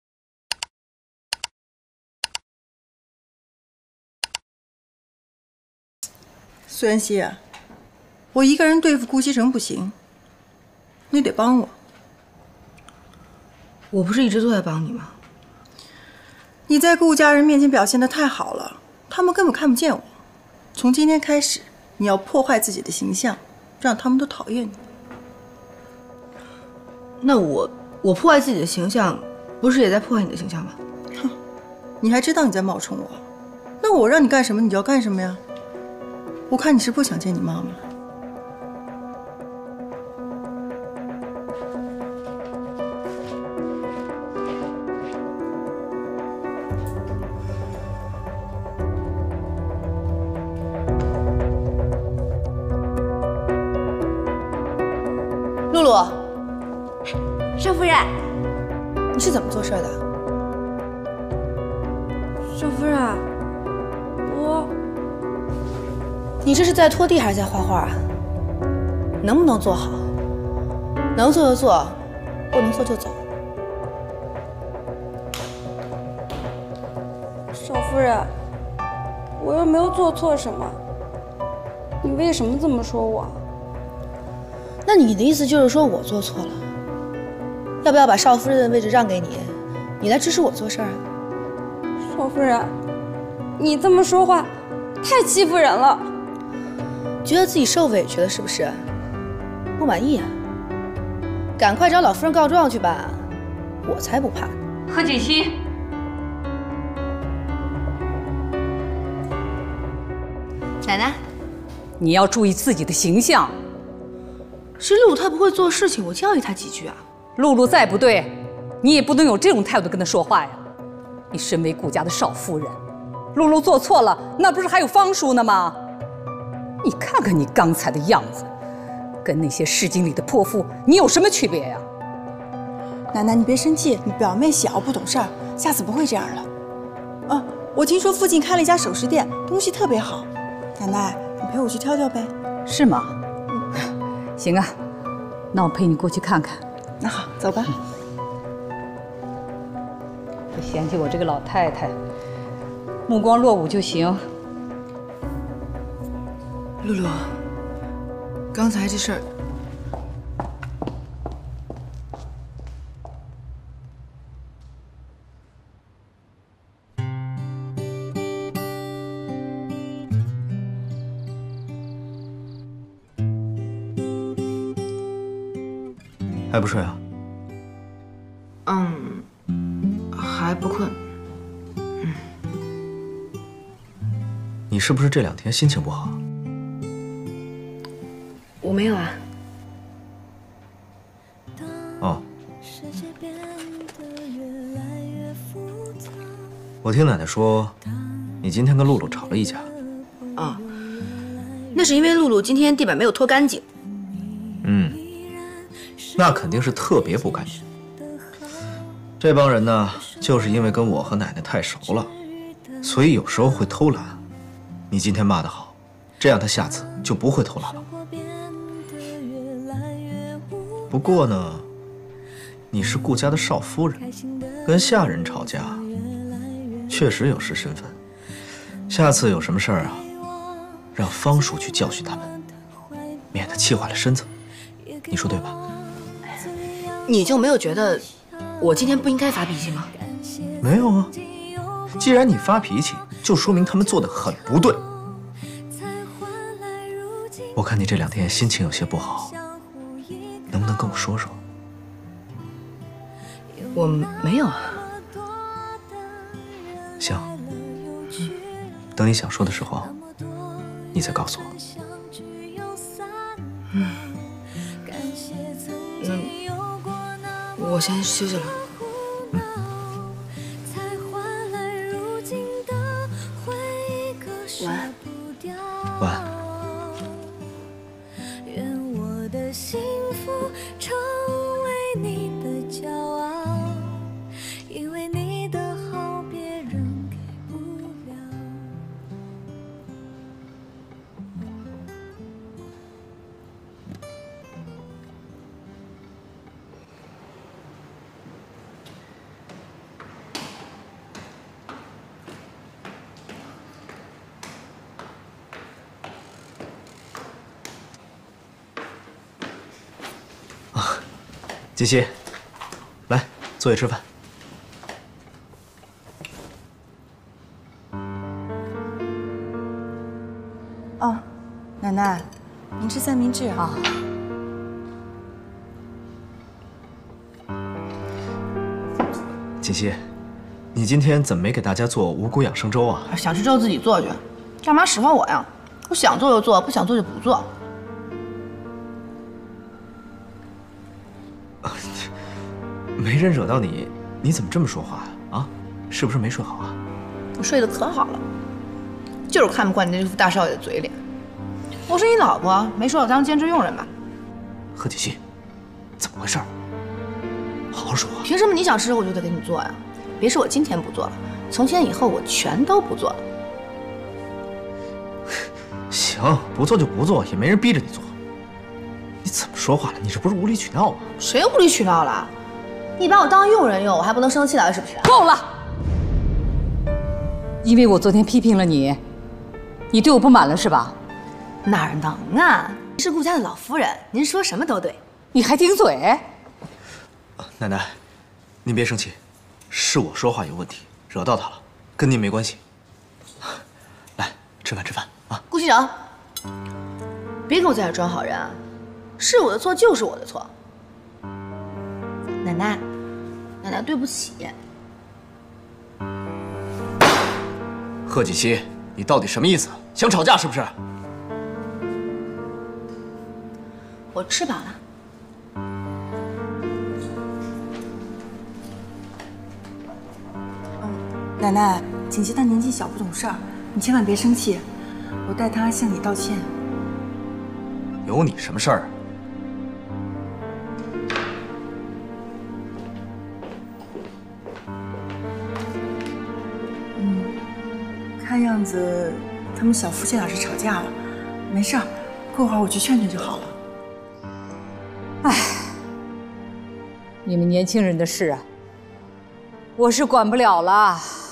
灰姑娘故意跟奶奶頂嘴，想讓總裁對自己失望，哪料總裁只是微笑著安撫她！💕Chinese Drama
【劇情簡介】該劇講述了灰姑娘替雙胞胎姐姐嫁給霸道總裁后，二人之間產生的甜蜜情緣。
 💓該視頻片段來源於電視劇  4TSTQ 19